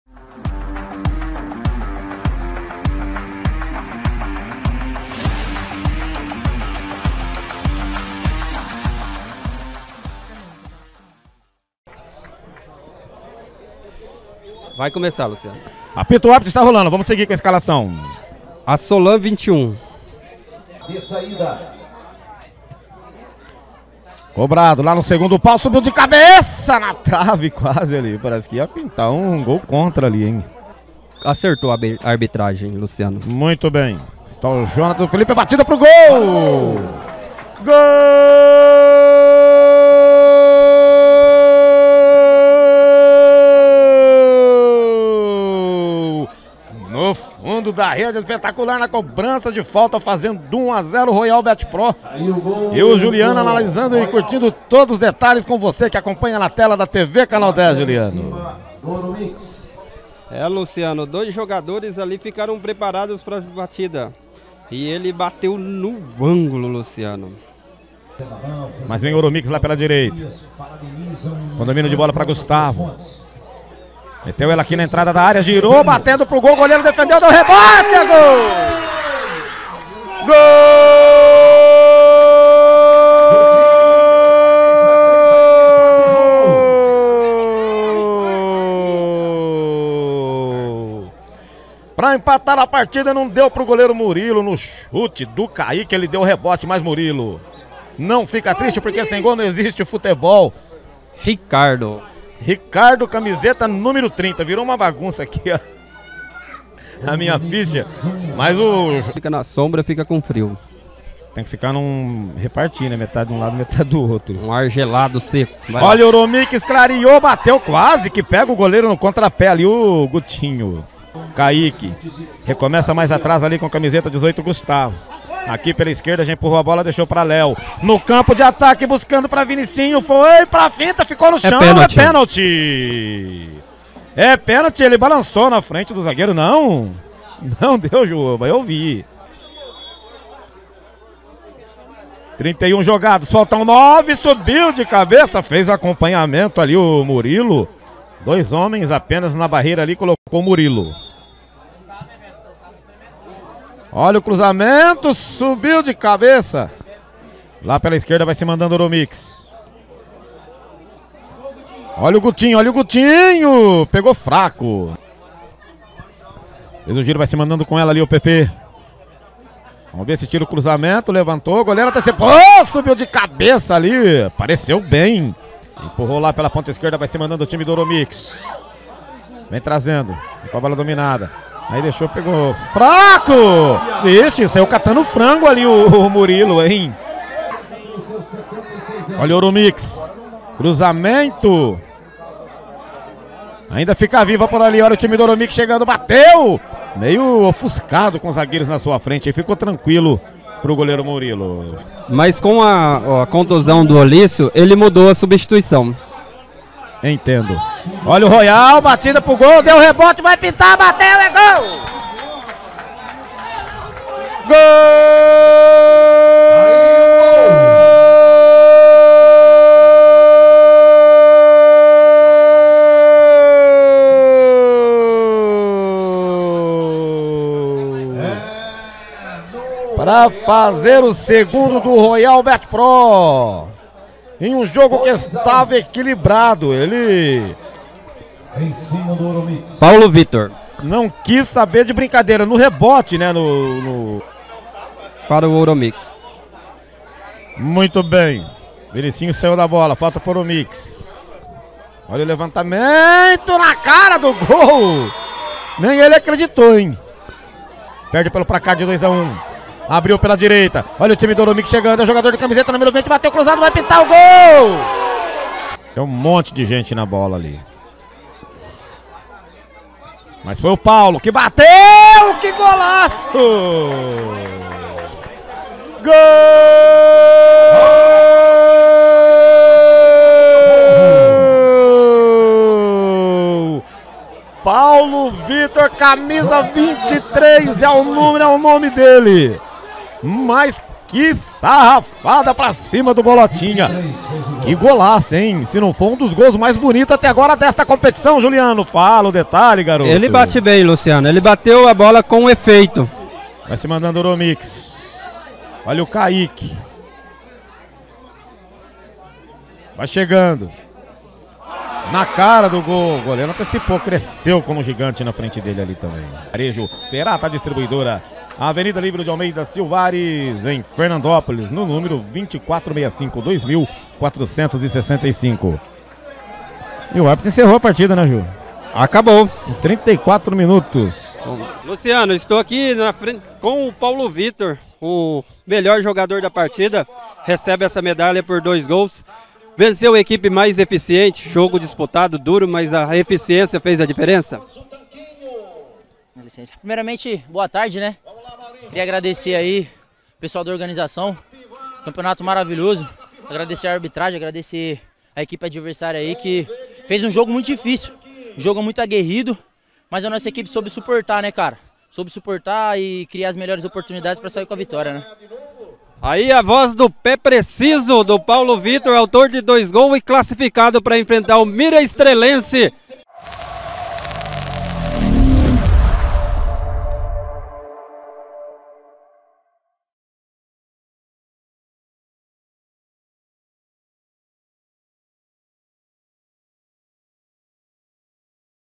Royal BetPro FC x Ouromix - Copa 24 de Junho
O adversário da Esportiva Miraestrelense por sua vez garantiu a sua classificação para a semifinal da Copa 24 de Junho no tempo regulamentar, o segundo jogo do dia foi entre Royal BetPro FC de General Salgado e Ouromix de Iturama/MG onde a equipe salgadense saiu com a vitória.